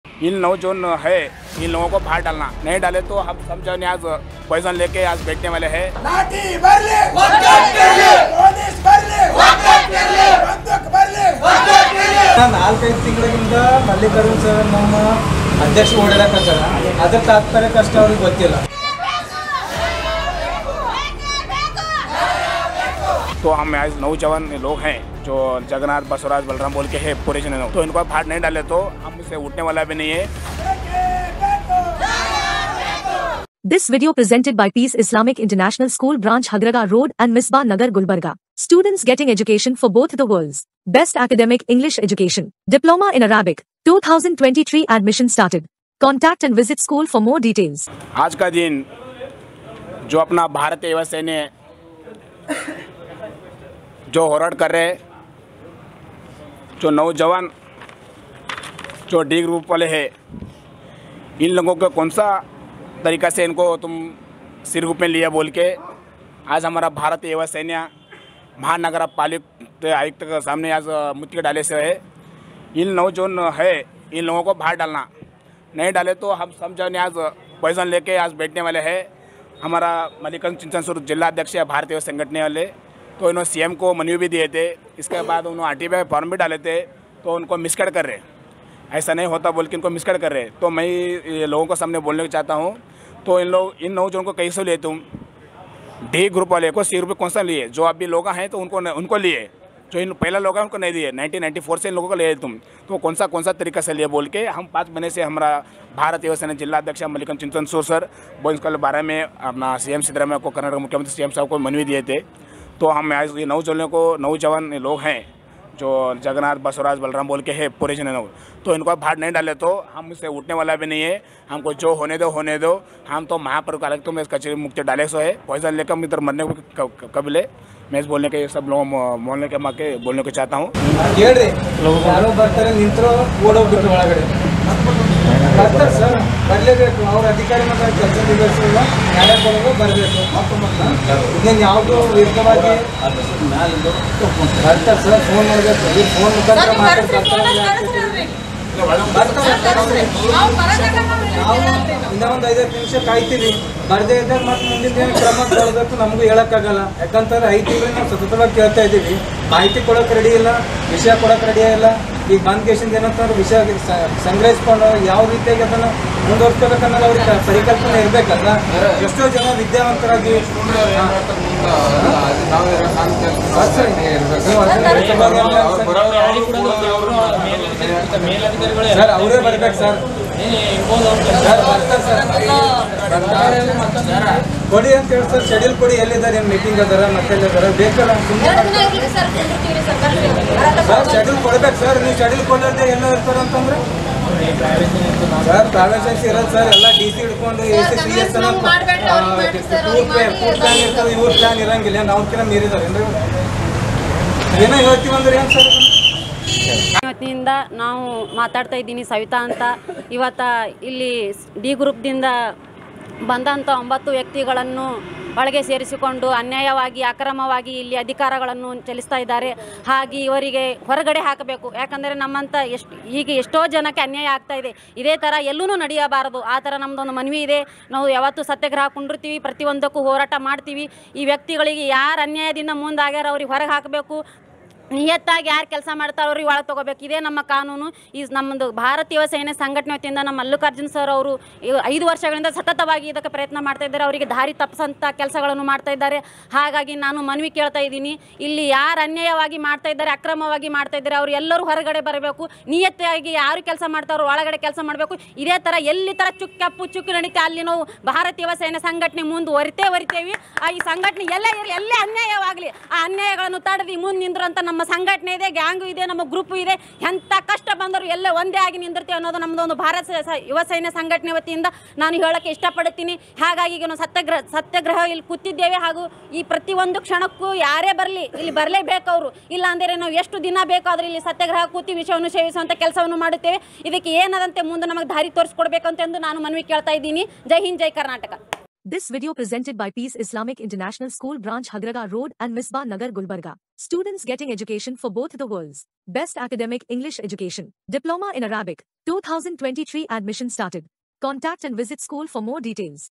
Bhartiya Yuva Sena Stages Protest, Demand Removal of 9 D Group Members Appointed in Mahanagar Paleke
Gulbarga Bhartiya Yuva Sena Stages Protest, Calls for Immediate Removal of 9 D Group Members Appointed in Kalaburagi Mahanagar Paleke, or Promises to Continue Protest